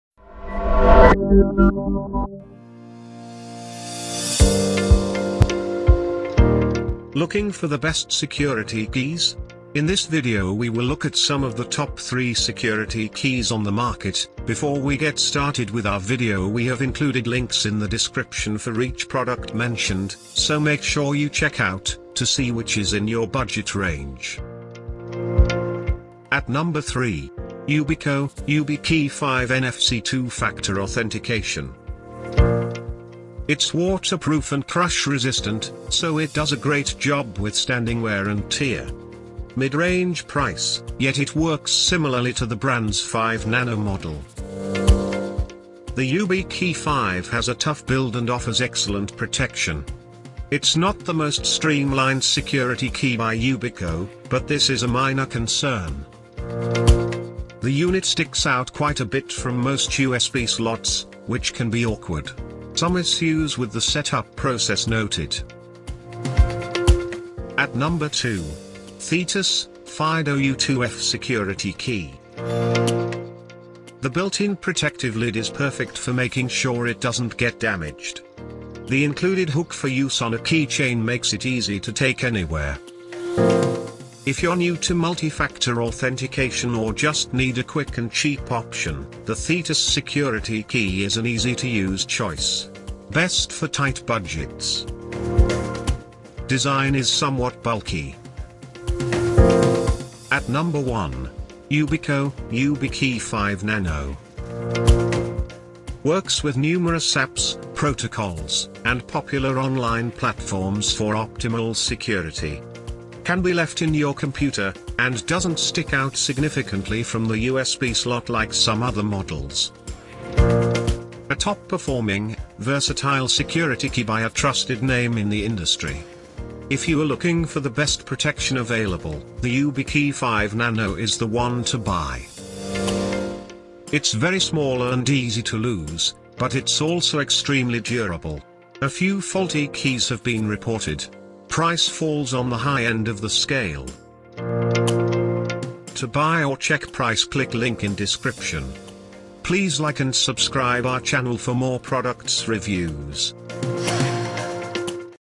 Best Security Keys for Multi-Factor Authentication in 2023
🌐 ➡️ Best Security Keys Reviews

1. Yubico YubiKey 5 Nano.

2. Thetis FIDO U2F Security Key.

3. Yubico YubiKey 5 NFC Two-Factor Authentication.
0:00 Top 3 Best Security Keys
0:26 Yubico YubiKey 5 NFC Two-Factor Authentication.
1:08 Thetis FIDO U2F Security Key.
1:40 Yubico YubiKey 5 Nano.